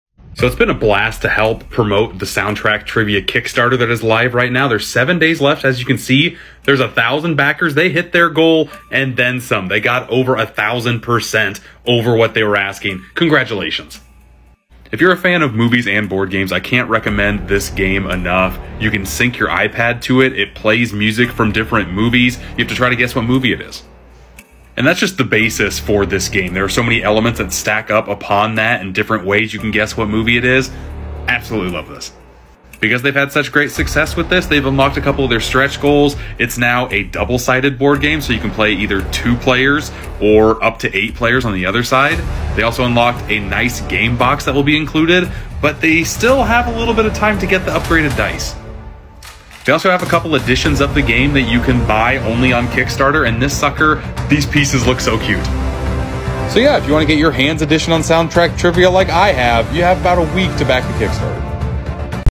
This Movie Soundtrack Board Game Is Crushing It On Kickstarter
If you love movies and board games than its a good thing you found this video.